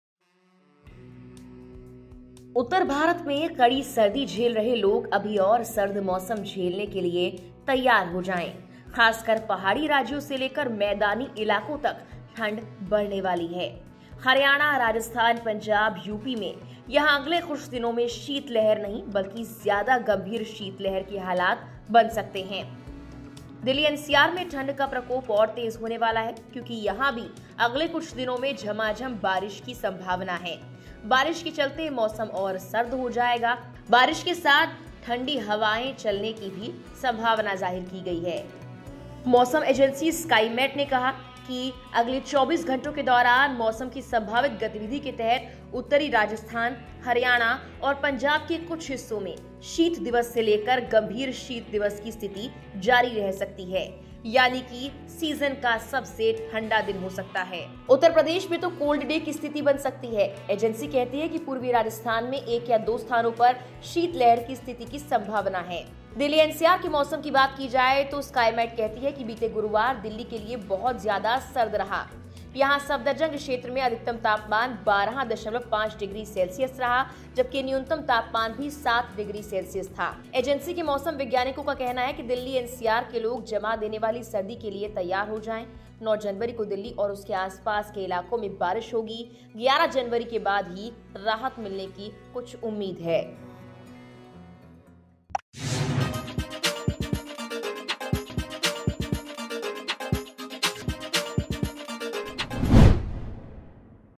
दिल्‍ली-NCR जमा देने वाली सर्दी के लिए हो जाएं तैयार, बारिश से बढ़ेगी सर्दी | Lokmat Hindi
People facing harsh winter in North India should get ready to face more cold weather. Cold is going to increase from the hilly states to the plains, especially in Rajasthan, Haryana, Punjab, Uttar Pradesh. Here, in the next few days, there may be conditions of severe cold wave and not cold wave.